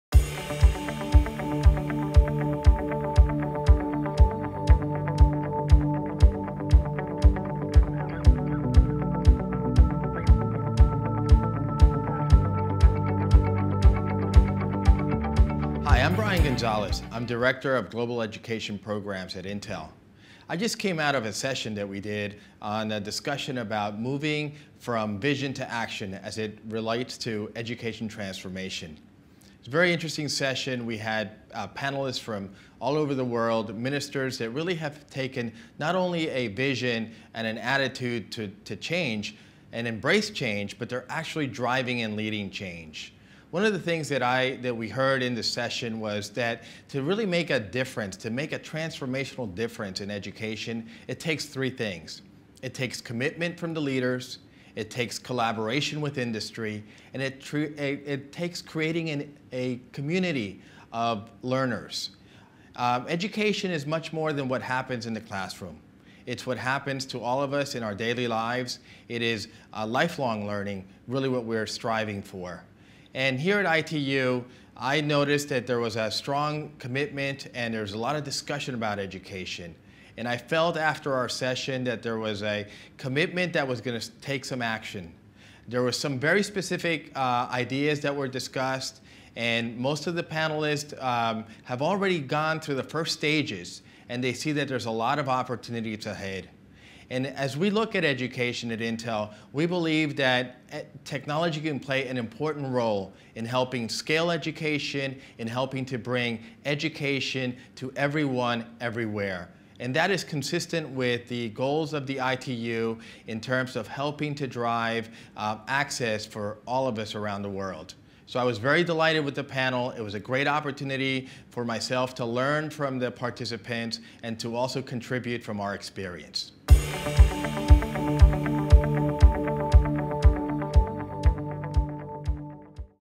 Brian G Gonzalez - Session Summary ITU Telecom World 2013.
Interview with Brian G Gonzalez Interview ITU Telecom World 2013.